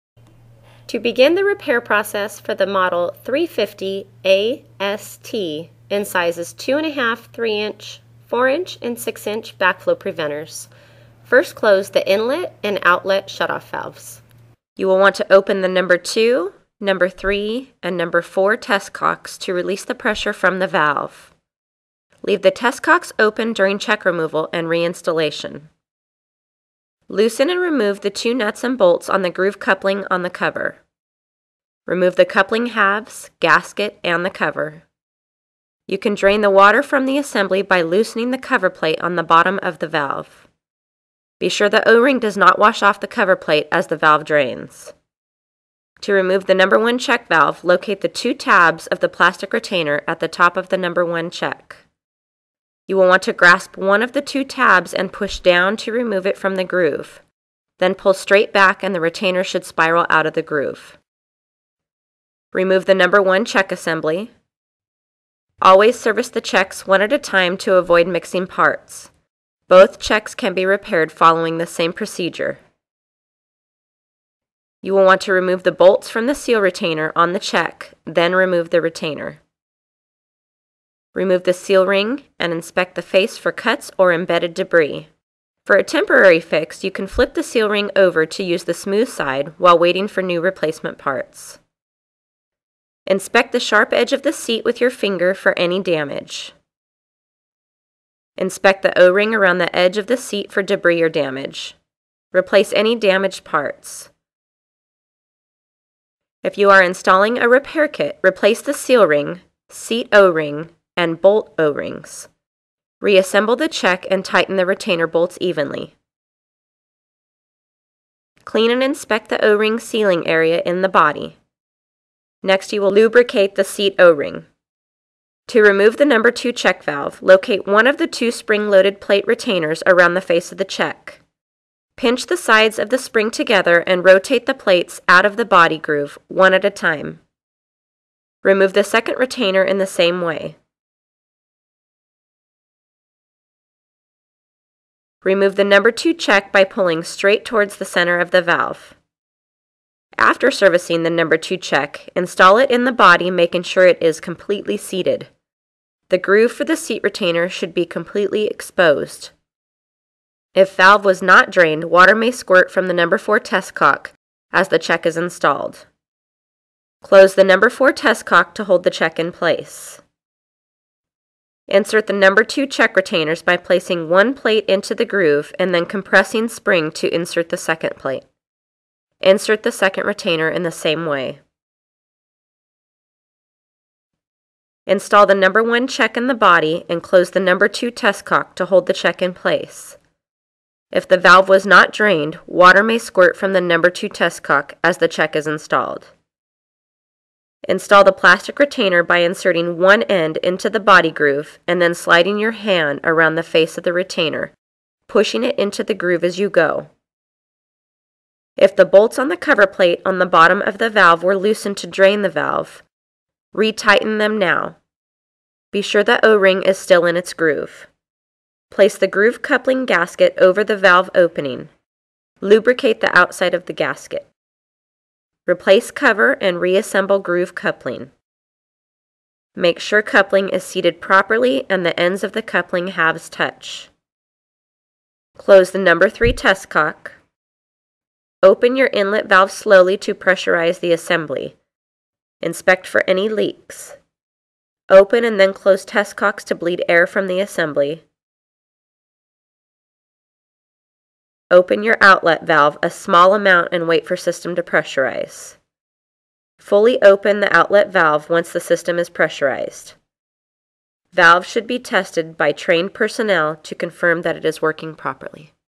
Repairing a Zurn Wilkins 350AST Double Check Valve Assembly
Watch as we walk you through, step-by-step, how to repair a Zurn Wilkins 350AST double check valve assembly backflow preventer.

*Note this video is more than 4 years old and may include some out-of-date information. Please refer to Zurn.com for current information and instructions.*